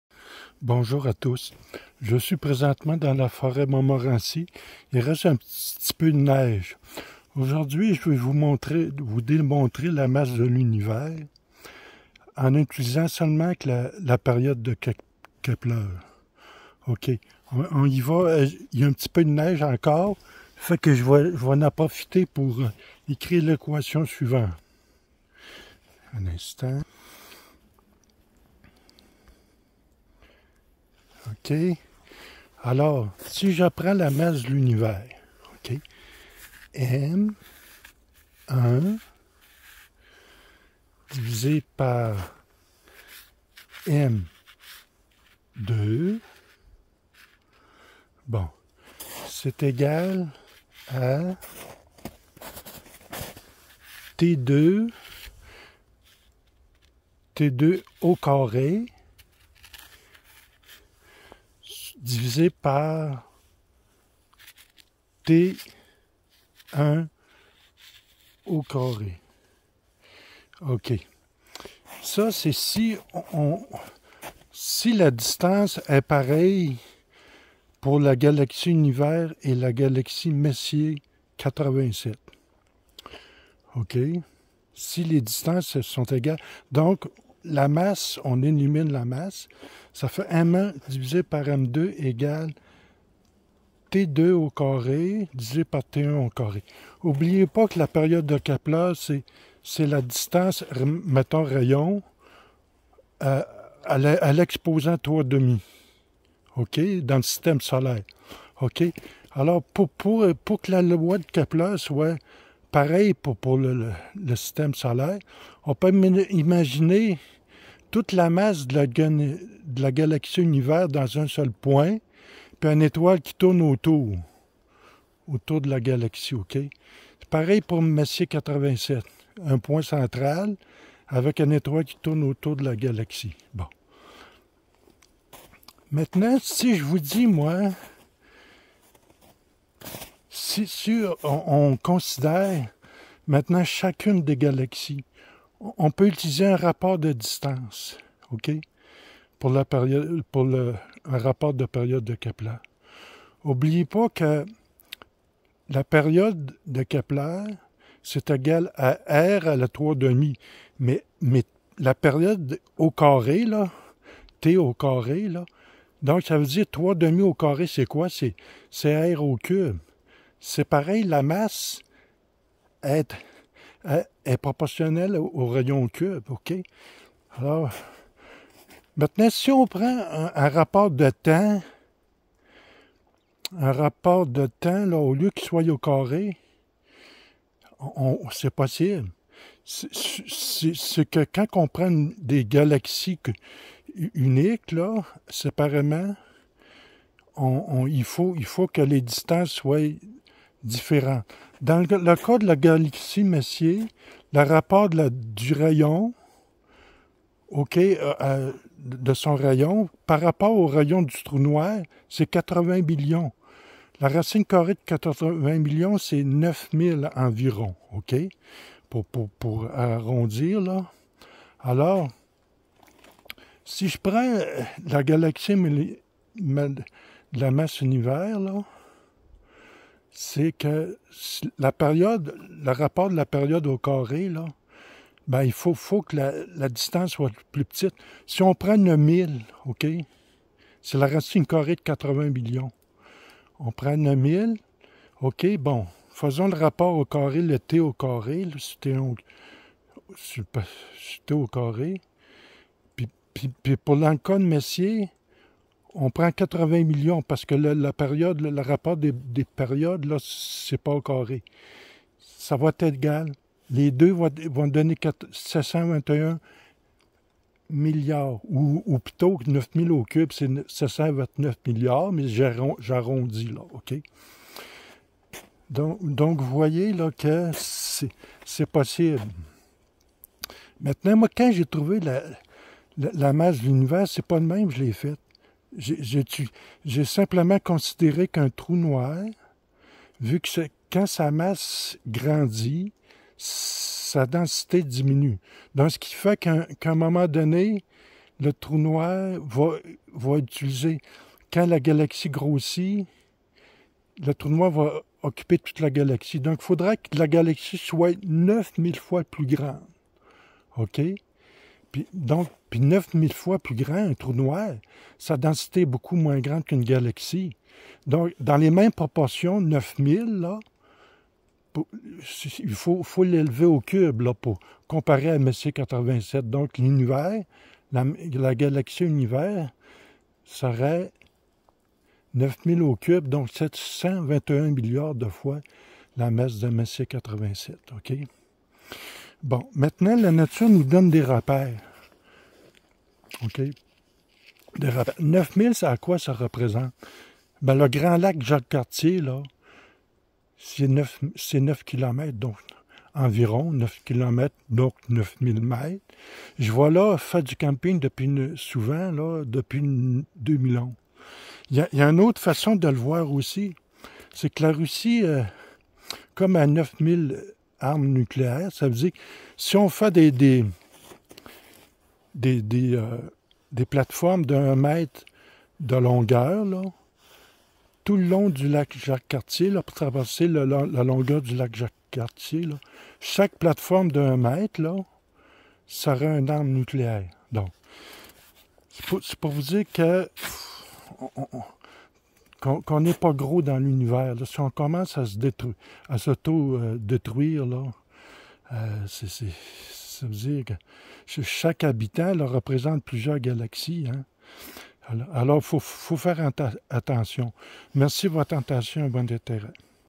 Démonstration de la masse de l'Univers avec la période de Képler
On peut avec la période de Kepler démontrer la masse de l'Univers, en effet si on prend la galaxie Univers  et la galaxie Messier 87 et qu'on considère une même distance, alors la loi suivante s'applique;
M1/M2 = [(T2)^2]/[(T1)^2]
si on prend chaque galaxie séparément cela peut aussi égaler le rapport (T2)/(T2) parce que l'on peut faire le lien entre deux temps T et deux distances. Pour la galaxie Messier 87 il faut donc un rapport de distance plus grande soit 8o millions environ qui est le rapport de distance entre le rayon de Messier 87 et le rayon de son trou noir. puis pour la galaxie Univers il faudra que le rapport des distances soit plus petit, soir la racine carré de 80 millions qui est environ 9000, le rapport M1/M2 sera toujours de 729 milliards, j'avais calculé 721 milliards qui doit ètre plus précis.
9000 est un repère qui représente le nombre de fois qu'il faut augmenter le rayon de la galaxie Messier 87 pour que. son trou noir occupe toute la galaxie étant donner que plus un trou noir est massif et plus sa densité est faible.